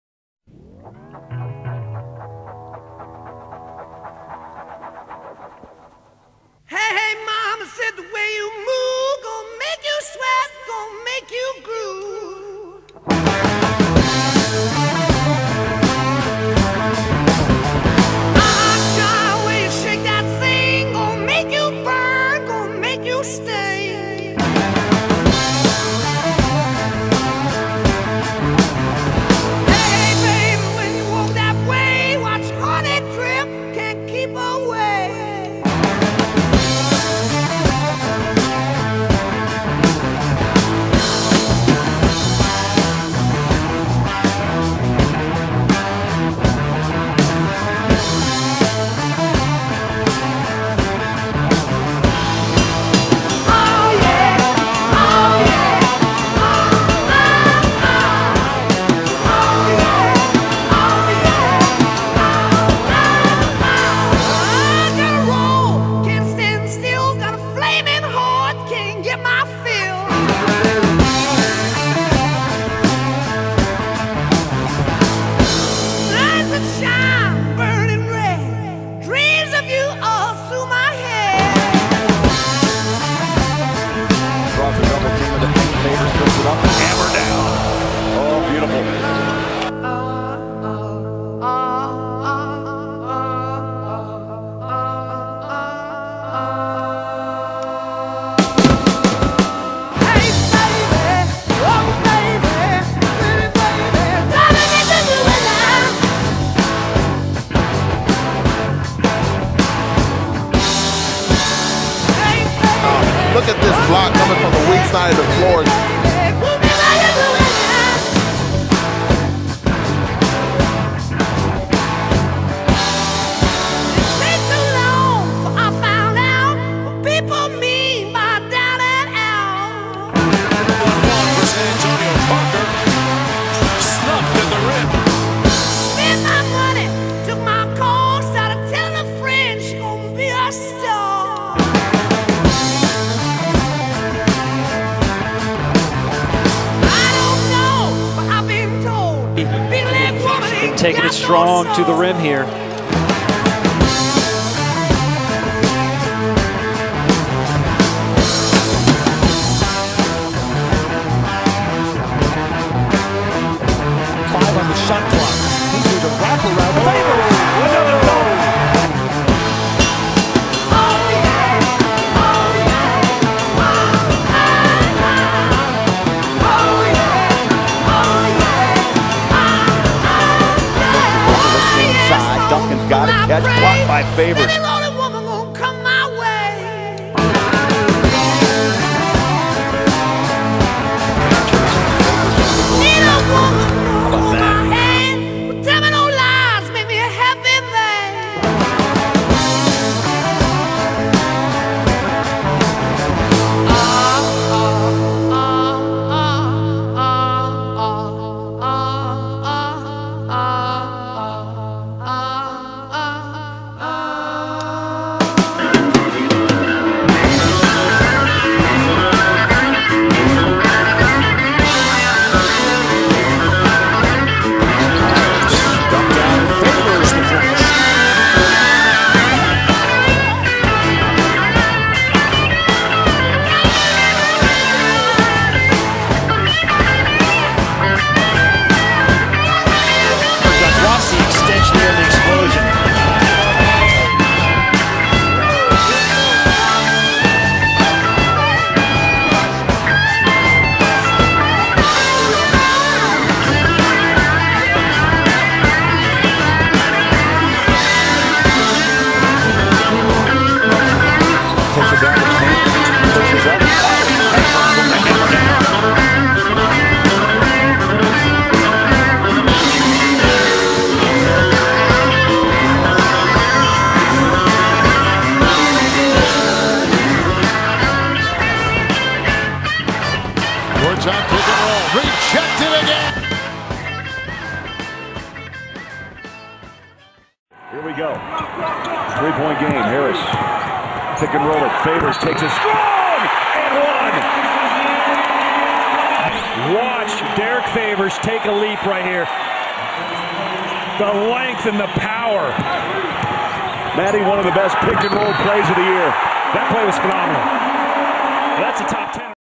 Derrick Favors 11-12 Season Mix
Highlights from Derrick Favors 2nd season with the Utah Jazz. Favors averaged 8.8 ppg, 6.5 rpg, 1.0 bpg in just 21.2 mpg. Favors showed a lot of promise this season especially on the defensive end. The future is bright with the young talent on the jazz.